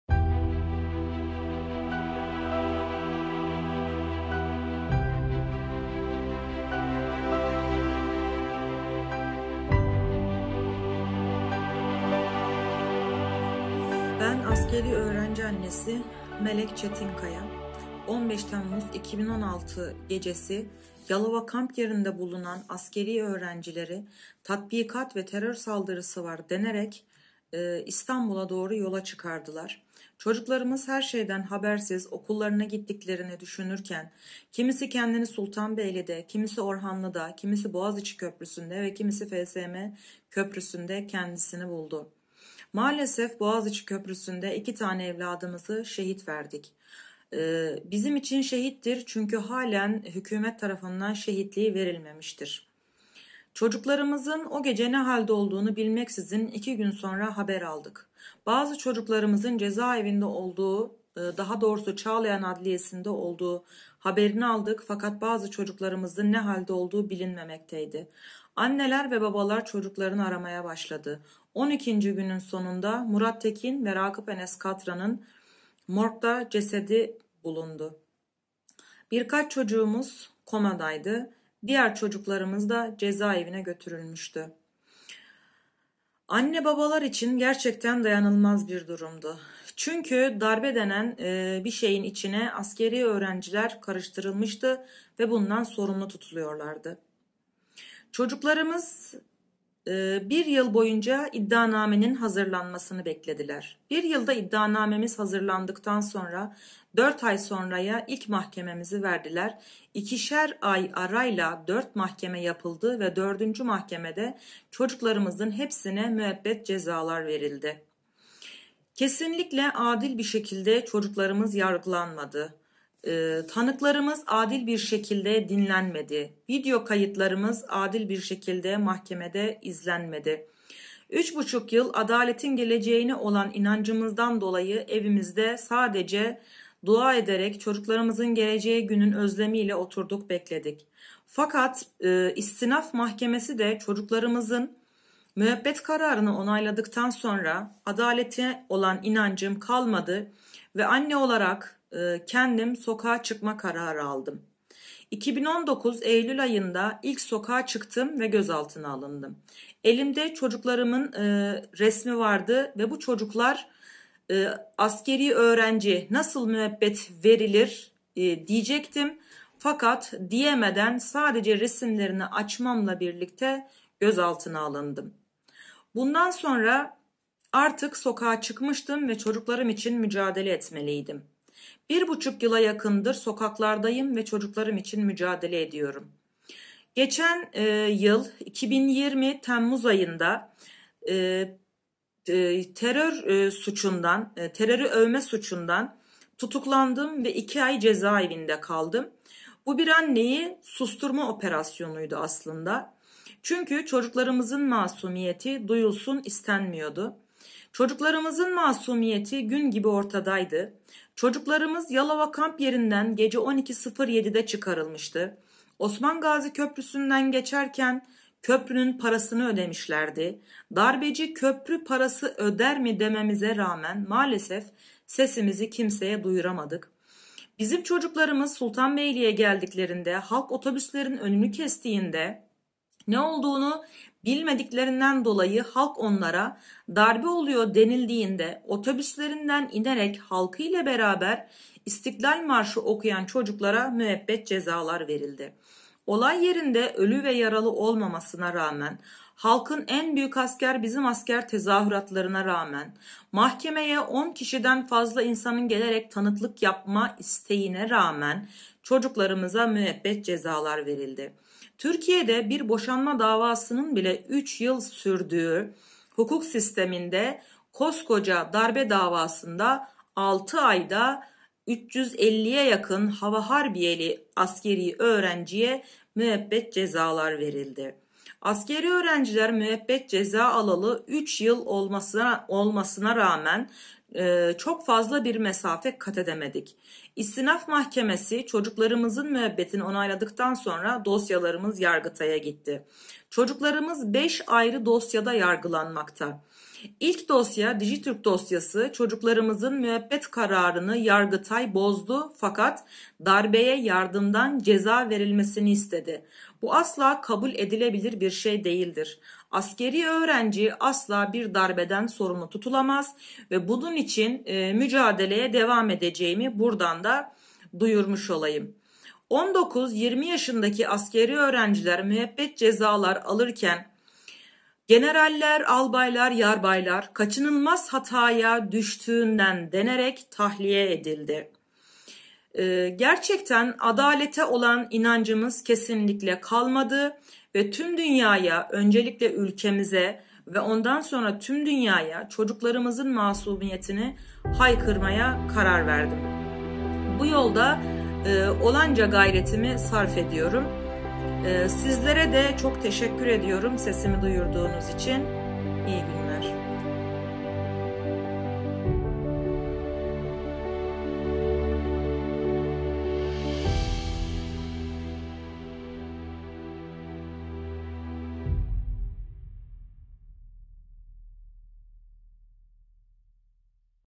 Melek Çetinkaya, fighting for the innocents: Military students imprisoned for life!
Melek Çetinkaya, whose son, a military school student, was wrongfully accused of taking part during the "Staged Coup" in Istanbul on July 15, 2016  and was sentenced to life imprisonment. Today, she's not only is fighting for her son but freedom for all the military students whom she treat today like her own sons. She is fighting for justice for the victims of torture and all the innocent people in her country of Turkey and sharing their voice with the International Community

She is an amazing example of courage and resilience for all military families and moms all over the world.